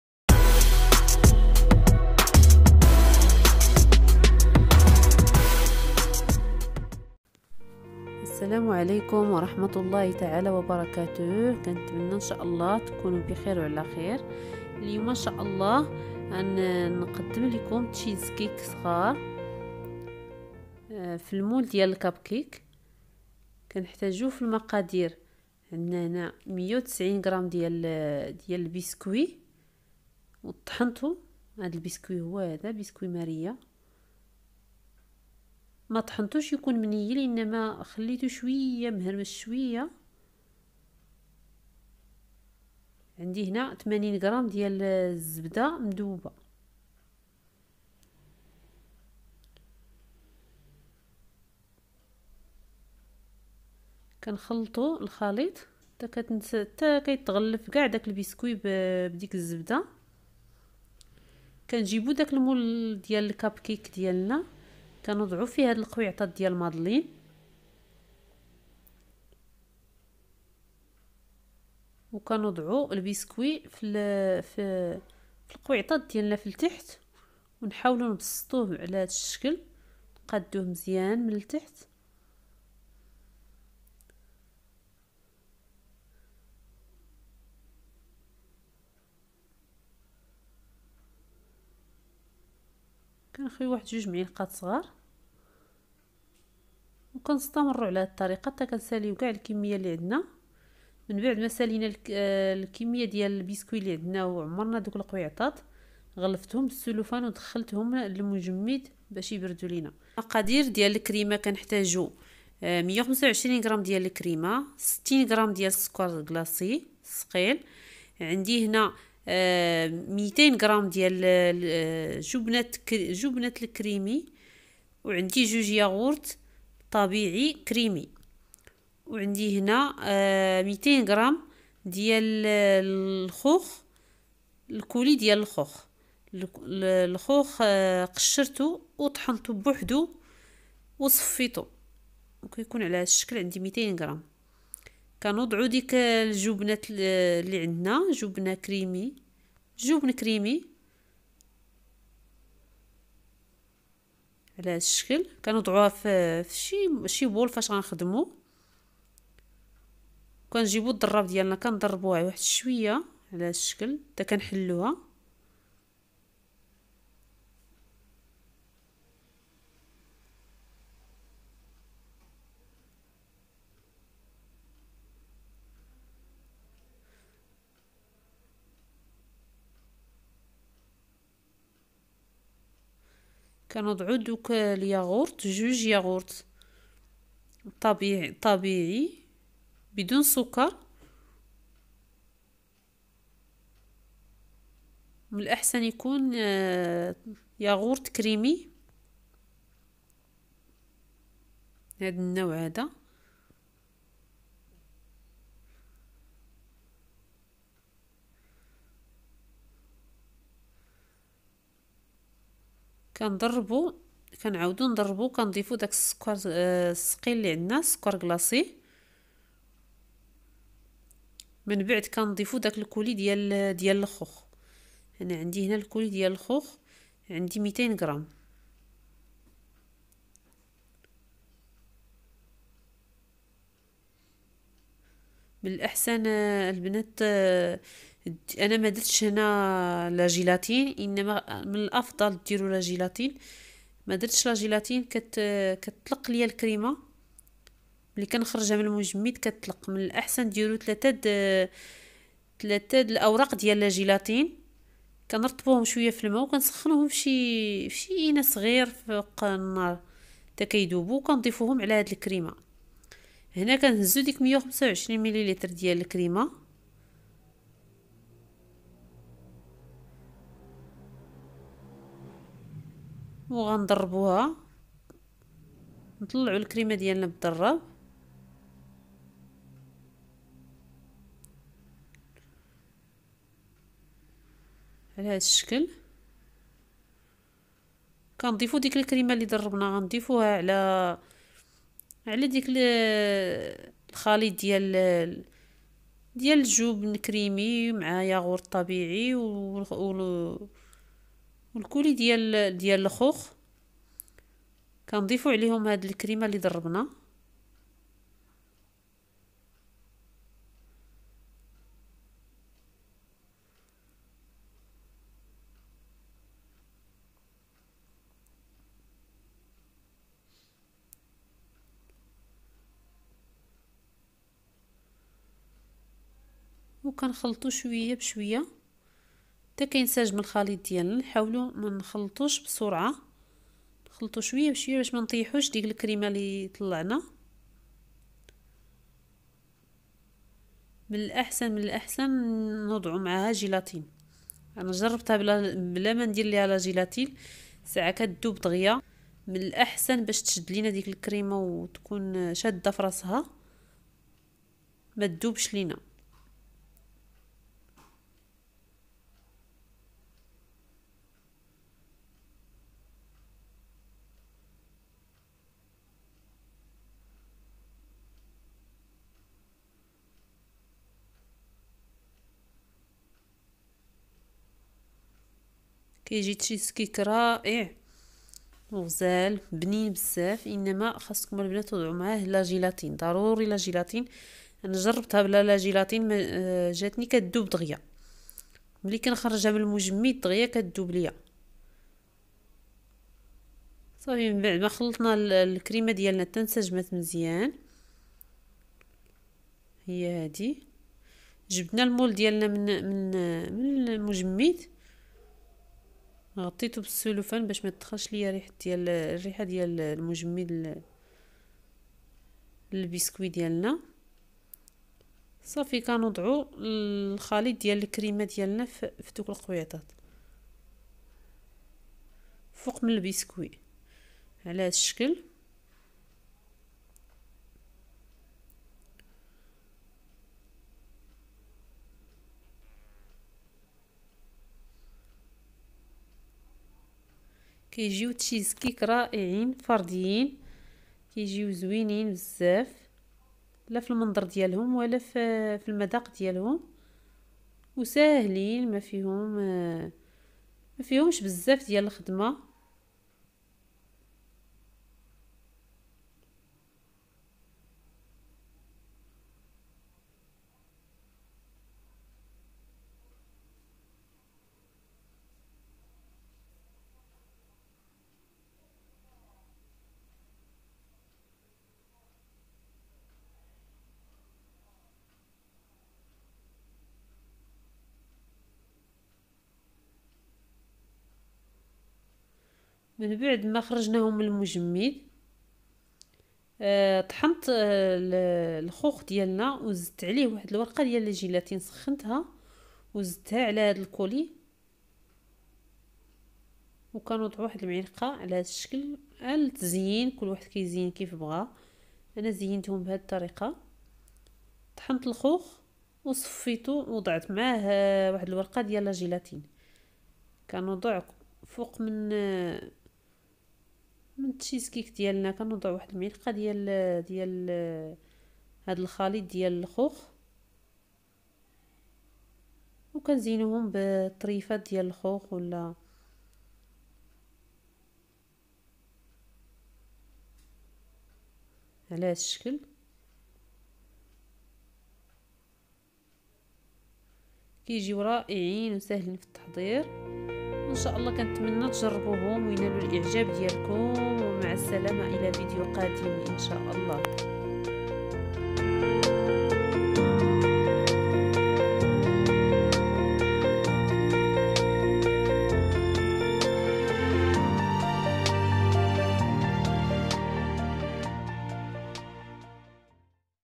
ميني شيزكيك فرديين بالخوخ / ساهلين فالتحضير / بدون فرن/ و المذاق روعة
ميني شيزكيك

Mini cheesecake

المقادير :